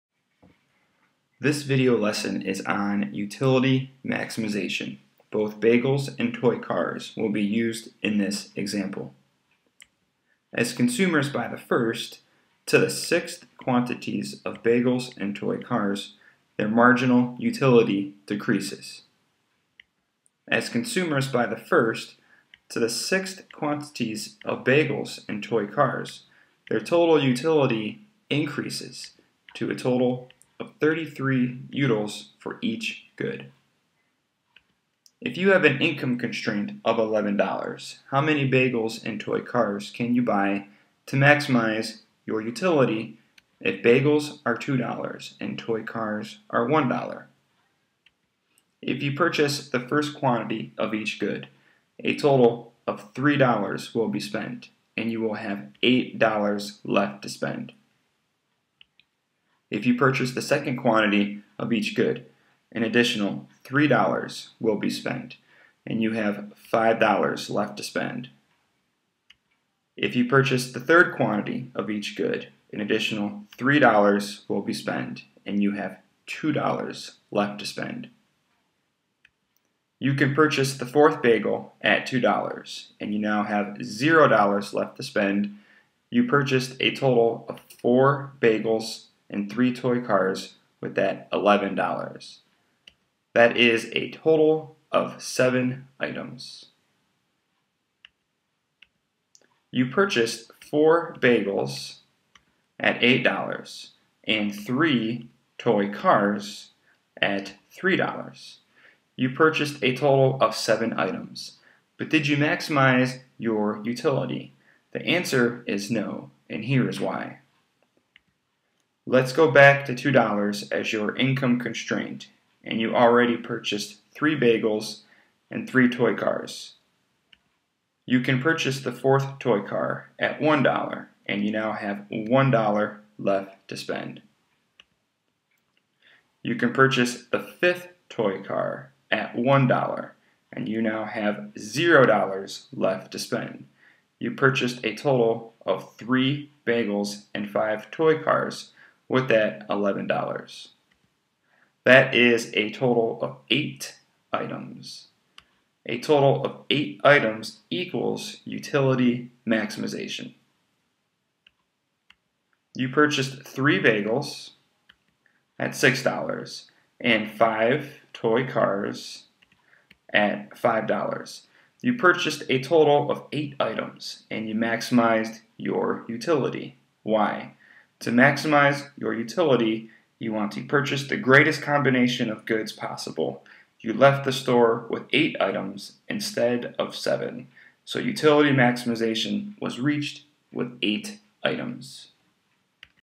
Utility Maximization
This video lesson is on utility maximization.  To maximize your utility, you want to buy the greatest combination of goods with the income that you have to spend.